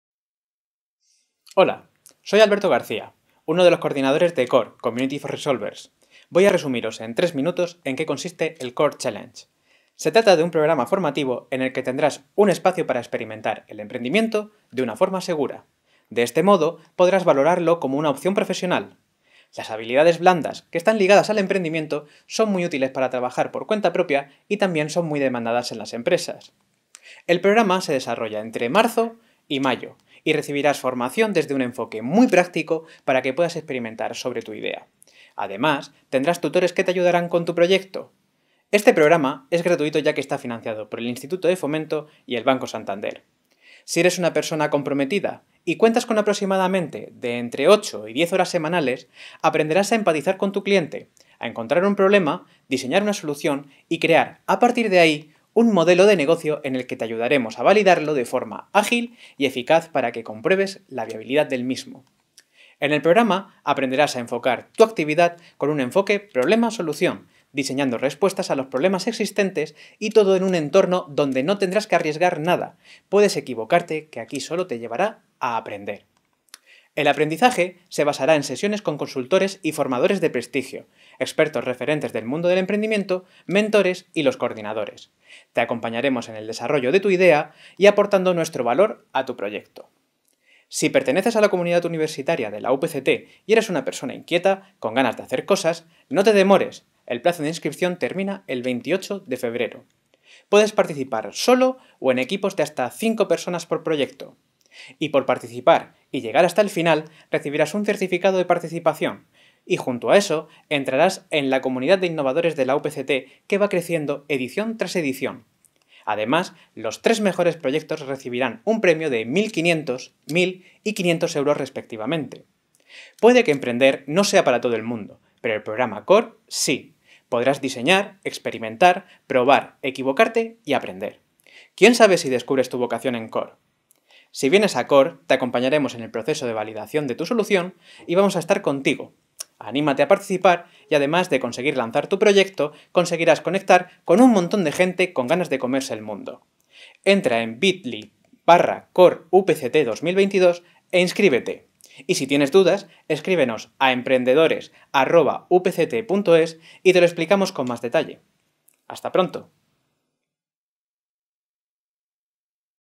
Plazo hasta final de mes para participar en el programa de emprendimiento CORE
Nueva oportunidad para desarrollar ideas de negocio. La UPCT lanza una nueva convocatoria de su programa CORE (Community of Resolvers), con formación gratuita y principalmente online en emprendimiento y premios económicos para los tres mejores proyectos.

Puede participar en el programa cualquier persona, sola o en equipos de hasta 5 personas. Durante el programa, que se realiza entre marzo y mayo, se recibirá formación práctica, con expertos del emprendimiento y mentores. Los contenidos tienen su base en las metodologías ágiles como el Design Thinking, el Lean Startup (para tener un enfoque problema-solución centrado en el cliente) junto con habilidades blandas como comunicación, o el trabajo en equipo. Habilidades útiles tanto a nivel académico, como profesional y personal.

La preinscripción se realiza a través de este enlace de Santander X. El plazo finaliza el 28 de febrero.

El programa consta de dos líneas. Por un lado, el concurso o 'Core challenges', abierto a toda la comunidad universitaria y a egresados, consiste en presentar una solución a un problema o necesidad en uno de los 24 retos propuestos vinculados a las nuevas tecnologías emergentes, el diseño, la Industria 4.0 y los nuevos procesos de gestión.

Los tres mejores proyectos recibirán un premio de 1.500, 1.000 y 500 euros respectivamente. Con o sin idea de proyecto, se puede participar en el programa, recibir la formación y crear y desarrollar una idea de negocio y propuesta de valor.

La segunda línea contempla una modalidad de formación gratuita a través de talleres, abierta a toda la sociedad.

El pasado año, en el I Call to Solve, resultaron ganadores los proyectos 'Perspectiva Z', de participación ciudadana, otro sobre condensadores masivos de humedad atmosférica y un tercero sobre una turbina hidrocinética.

Más de medio centenar de personas se inscribieron en la pasada edición de este programa.